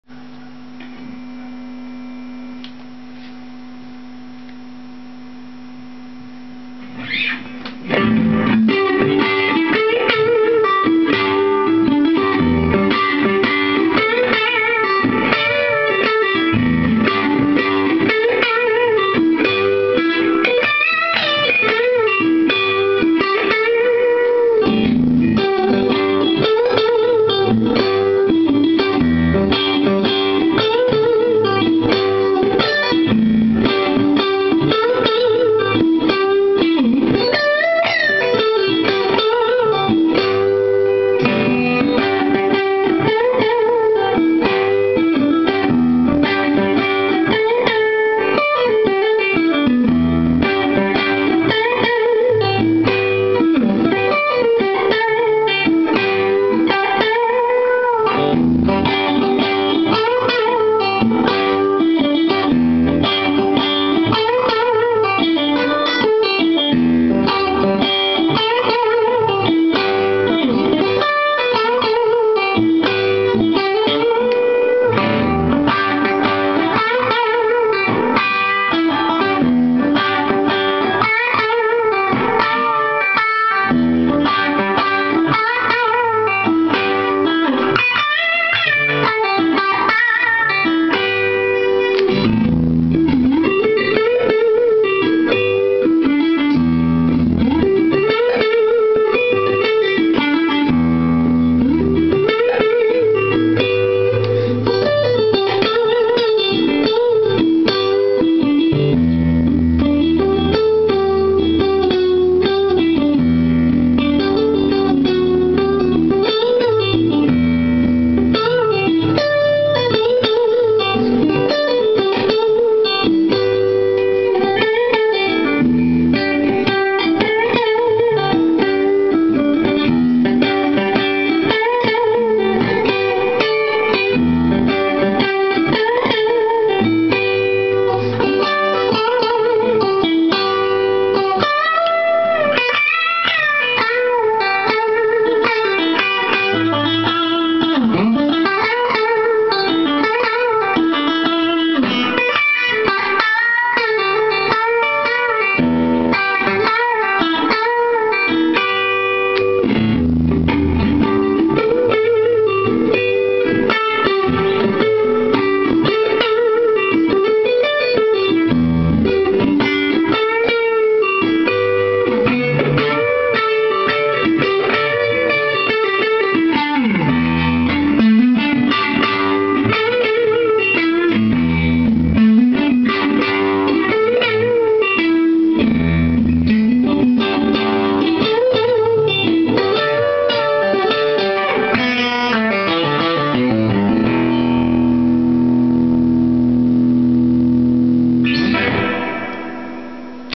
Ardizzi Pickups Special Tone Test 2
Fender Stratocaster MIM + Ardizzi Pickps Special Tone + VoxAC30C2 Top Boost Channel